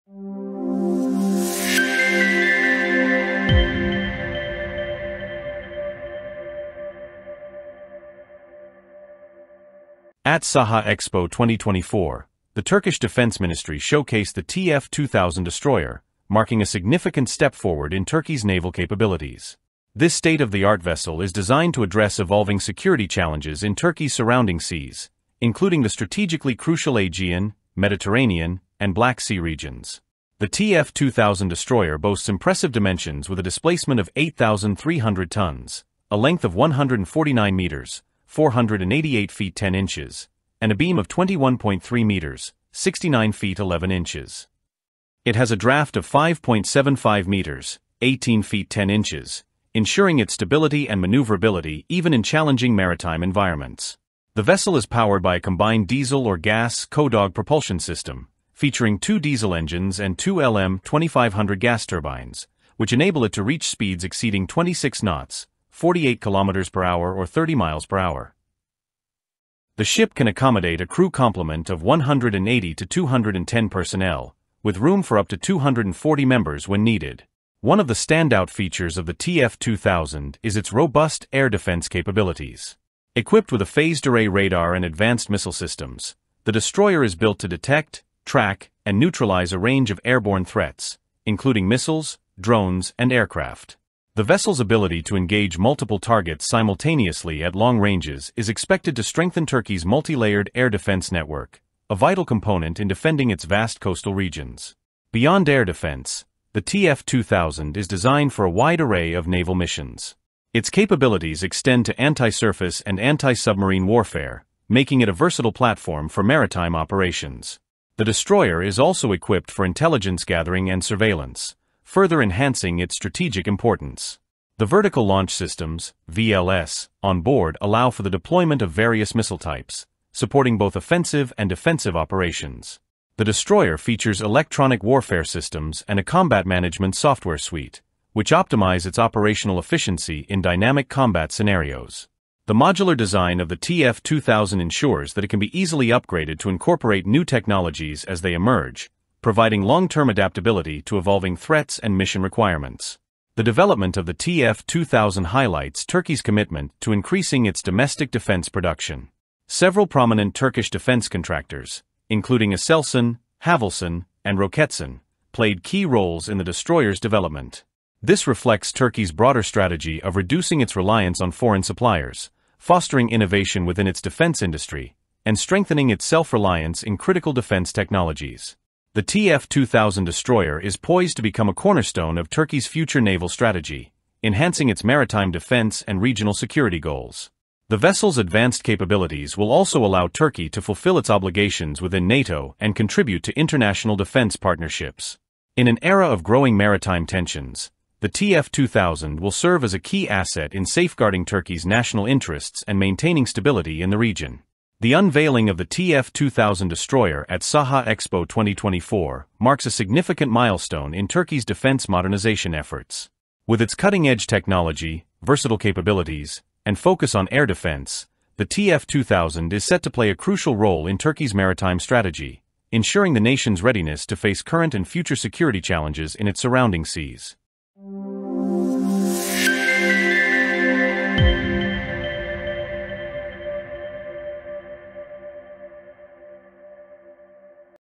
Turkiye new TF 2000 destroyer with Air Defense capability at SAHA EXPO 2024
At SAHA EXPO 2024, the Turkish Defense Ministry unveiled the TF2000 destroyer, a modern naval vessel poised to enhance Turkey’s maritime defense capabilities. With a displacement of 8,300 tonnes and advanced air defense systems, the TF2000 is designed to address security challenges in the Aegean, Mediterranean, and Black Sea regions. Powered by a Combined Diesel or Gas (CODOG) propulsion system, this destroyer can reach speeds over 26 knots. Equipped with phased-array radar and vertical launch systems, it can detect and neutralize various airborne threats, making it a critical asset for Turkey’s layered air defense strategy. The destroyer’s modular design ensures adaptability to evolving technology and mission requirements, reflecting Turkey's focus on increasing domestic defense production. With significant contributions from Turkish contractors like ASELSAN and ROKETSAN, the TF2000 represents a leap forward in Turkey’s naval capabilities and strategic defense initiatives.